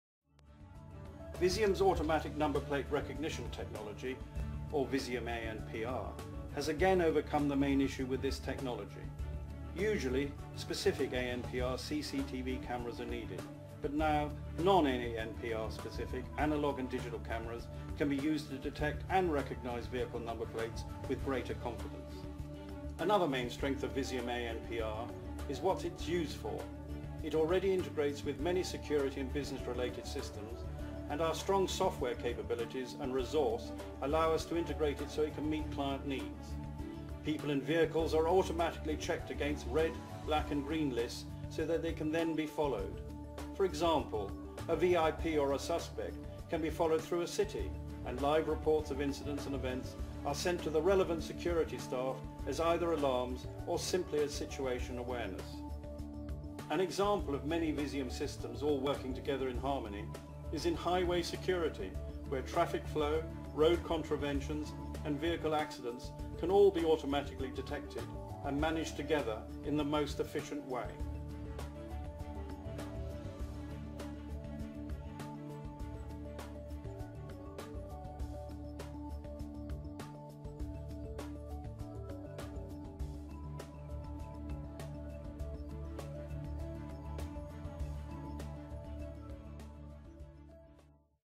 Traffic Enforcement System - Traffic CCTV
The visual appearance of this traffic CCTV camera operating automatically protecting all surroundings, displays to would-be traffic offenders the entire area is constantly watched by many traffic enforcement surveillance experts.